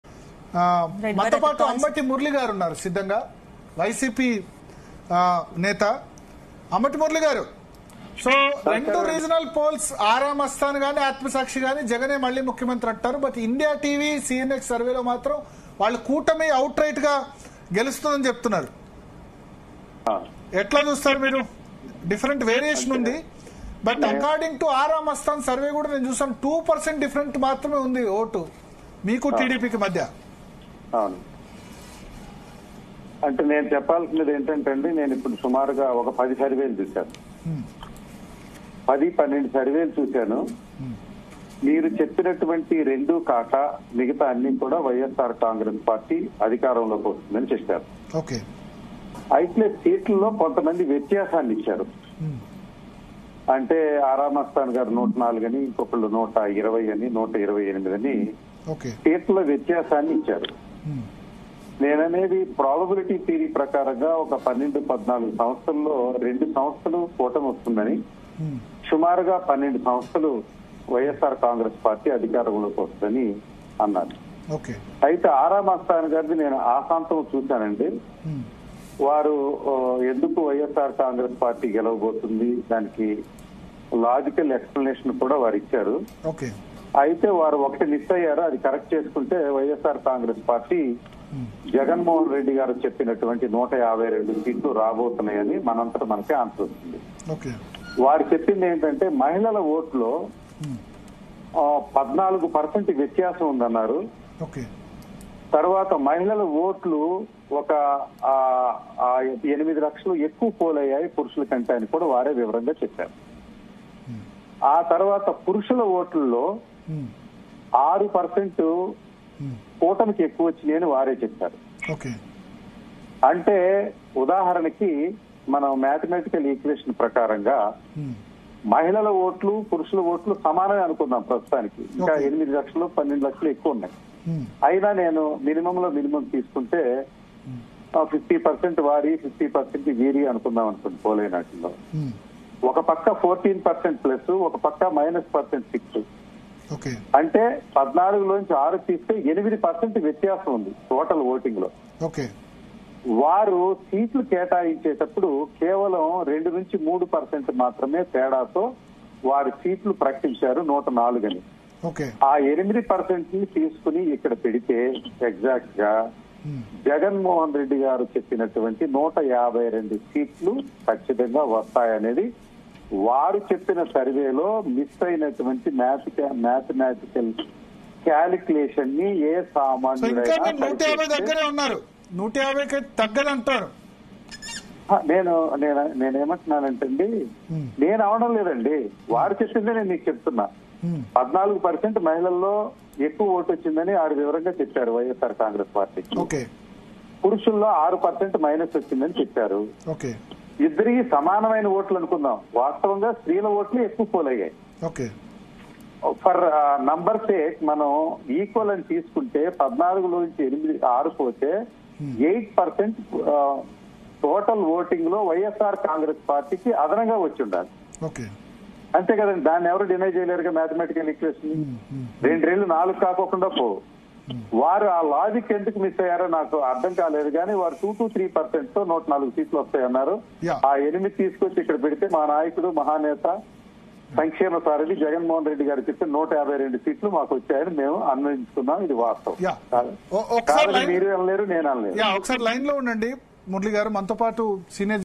Ambati Murali On Exit Poll Survey Reports | ఫిక్స్ అయిపోండి!! జగనన్నే సీఎం.. | ZEE News Telugu
ఫిక్స్ అయిపోండి!!  జగనన్నే సీఎం.. | Ambati Murali On Exit Poll Survey Reports | ZEE News Telugu